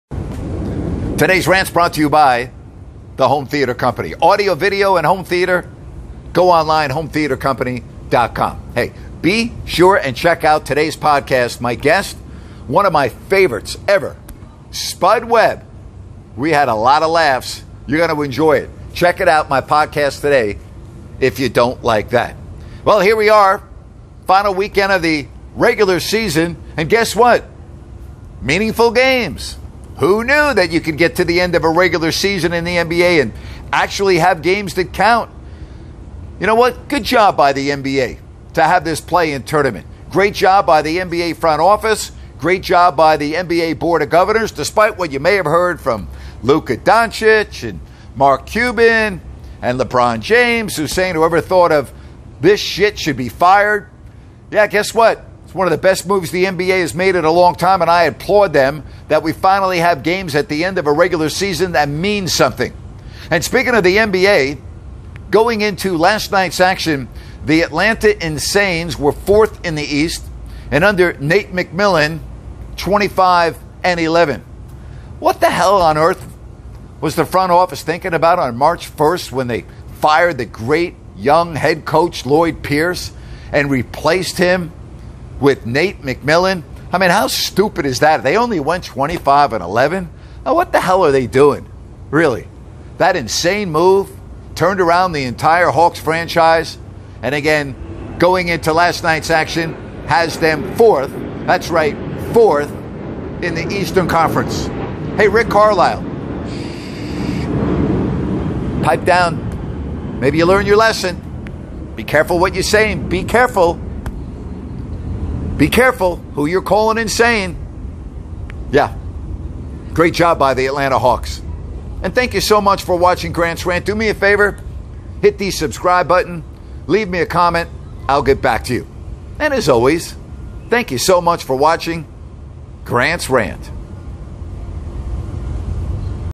Grant's Rant: 5-14-21 NBA Play In Tournament means these games count Lebron!
Yes, this is going to be a fun weekend of basketball.  As a result of the play in tournament, players are going to have to hustle to get their teams into the playoffs.  And that means some folk are going to be happy this weekend and some are going to be not so happy.  Good job to the NBA for the play in tourney because the games mean something. And I have to acknowledge the Atlanta Hawks  who are currently fourth in the East after firing their head coach with a lot of dissent from Rick Carlisle.  Nate McMillan was the choice in the interim and he has done one helluva job.  Incredible.   I can't wait to see how far they can go.  All that said, looking forward to talking about the weekend next week as we head into the playoff season with every game counting.  Have some fun and enjoy the competition.